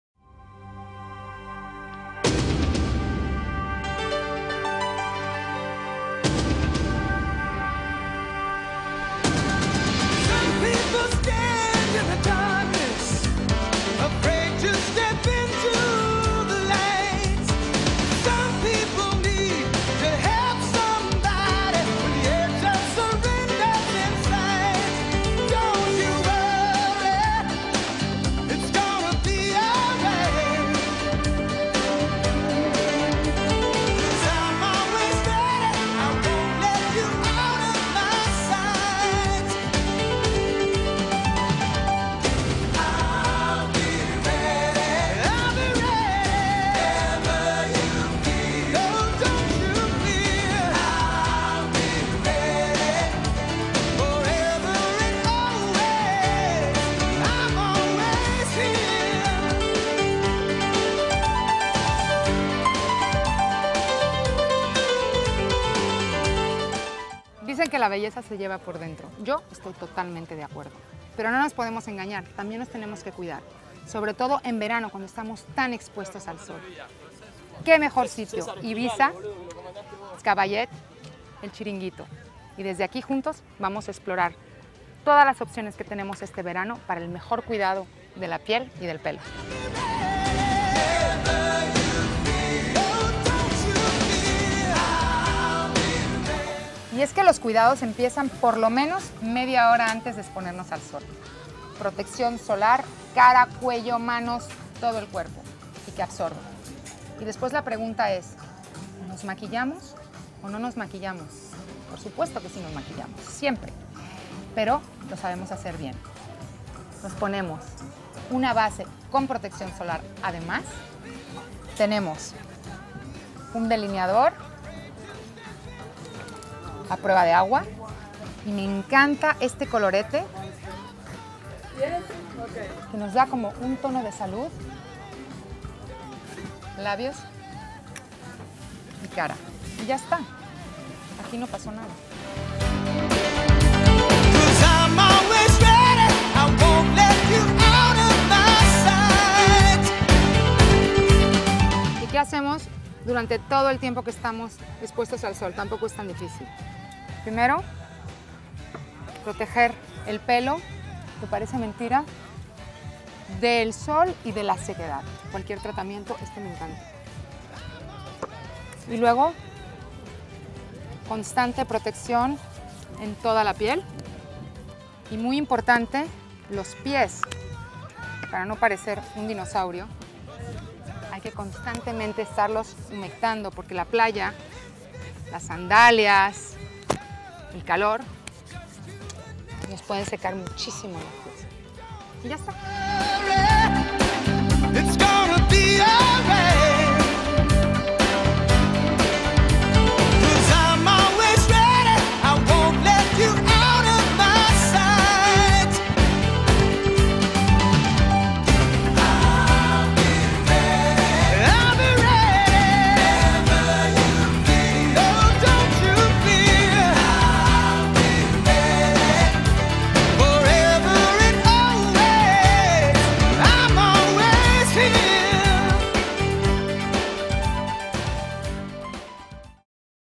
Adriana Abascal Blog: Beach Beauty SOS
Al rescate de la piel y pelo con los cuidados de beach beauty sos para no caer en la tragedia de freírnos en las playas en verano? Por que no!

Sin darnos cuenta y ante el entusiasmo de ponernos morenas, vivir el momento  disfrutando a tope del verano podemos estropearnos gravemente la piel y el pelo, y luego que? No compensa! Pero no hay que renunciar al ponernos morenas y estupendas y disfrutar, no te preocupes! Solo tenemos que hacer algunas cosillas a lo largo del dia...

Tampoco se trata de privarnos de nada simplemente de no olvidarnos de nuestra piel y pelo para que en septiembre cuando ya no estamos en la playa en bikini, no nos llegue la factura del intenso verano.

No estamos hablando de tratamientos al estilo Cleopatra pero para mi son varias pequeñas cosas que al final hacen la gran diferencia.
Los pequeños cuidados empiezan por lo menos una hora antes de exponernos al sol y el conceptos es  muy sencillo, hidratación, protección de los rayos UV y desde luego un toque sencillo de maquillaje a nadie le viene mal!

Por la mañana antes de salir al sol, Además de limpiar la piel, hidratar y ponernos protección mínimo del 15 nos podemos aplicar una base de maquillaje con protección solar que nos proteja y evite las manchas en la piel, un colorete a base de betabel, delineador y mascara de pestanas a prueba de agua es para mi la mejor pequeña rutina.

Ahora que ya estas lista para lucir estupenda en la playa y protegida del  sol tanto en el pelo con algún tratamiento que además de hidratarte te cobra del sol, y protección solar en la cara cuello, manos y cuerpo, mi mejor consejo es protección solar en contorno de ojos de clarins, y protección solar de cara por lo menos una vez cada hora, y si te banas aclarar el pelo y poner mas tratamiento capilar y por ultimo algún aceite o crema de pies, todo esto constantemente; los pies es otra de las partes del cuerpo que mas sufren durante el verano.

Después de un dia intenso de sol y mar lo mejor además de kerastasse para mi es la crema apres soliel mas crema de ojos y cara la que mas te guste y otra vez crema de manos y pies, todo esto de forma un tanto exagerada para no hundirnos y naufragar en las aguas del olvido.
Hacer todo esto parte de mis hábitos palyeros y sin darme cuenta y de forma automática me doy cuenta que  todo esto hace una gran diferencia desde luego!

Así que chicas a disfrutar la luz, el sol y el momento cuando se puede que la vida esta hecha de pequeños grandes momentos y solo se vive una vez!